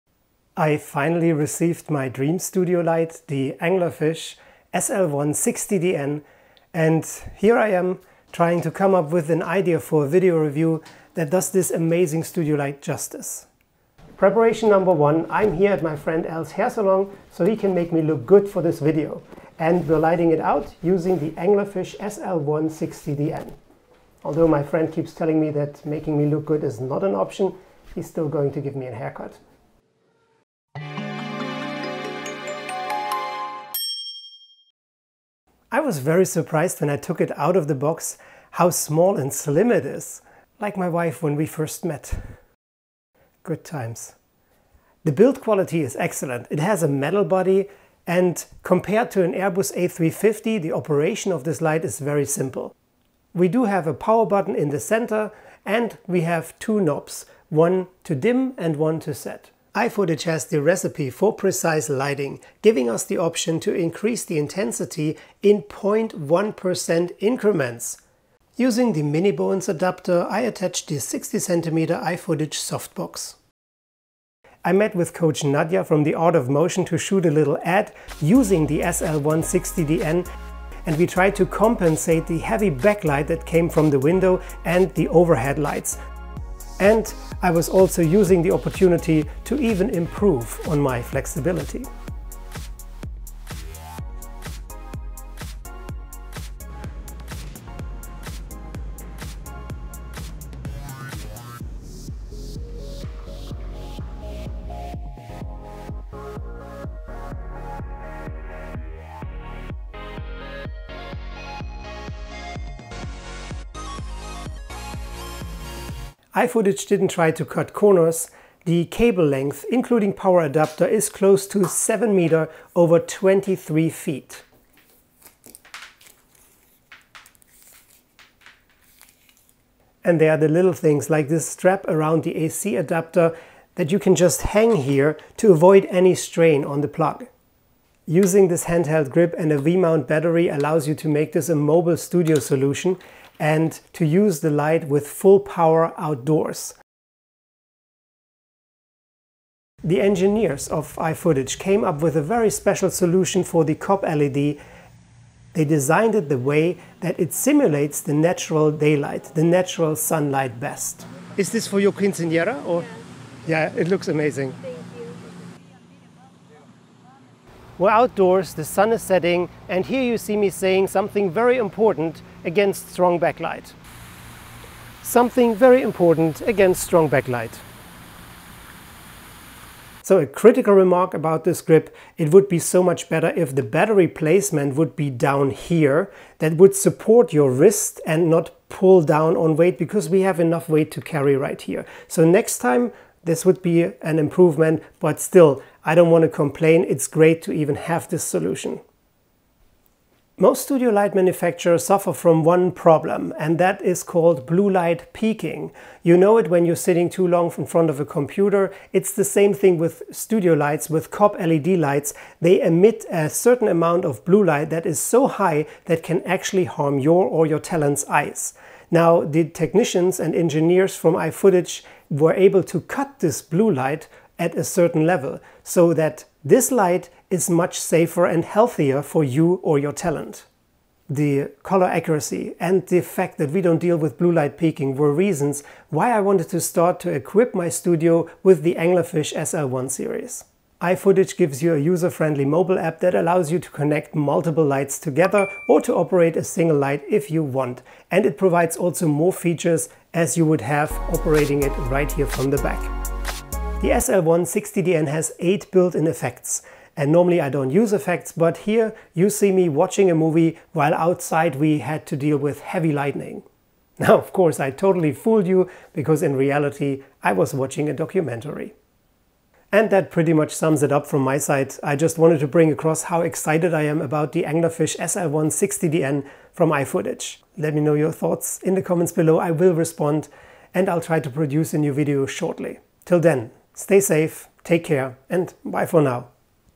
Best Mobile Studio Light - ifootage anglerfish SL1-60DN - review and test
Best Mobile Studio Light Anglerfish SL1-60DN - review and test
iFootage Studio Light - for creators photographers and filmmakers
On Amazon

Nadja  at The Art Of Motion  @nadzeyat
Alis at Hairstudio AM  @alis.m_hair

00:00  The Preparation -  Hairsalon AM
00:35  The Obvious
01:14  The Art Of Motion
01:56  The Extras
02:22  The Outdoors
03:19  The Blue Light
04:00  The Mobile App
04:14  The Effects

The build quality of the ifootage anglerfish SL1-60DN is really good. And the same goes for all accessories. The soft box has a triple diffuser layer and is really precise for perfect even lighting

BTC: bc1qmwme7dxsrek7h9z7vnrsf3en3mm4d9lq3zxvq3
ETH: 0x314F728aef158f00daF8FaAAE38E169e4ED1C86E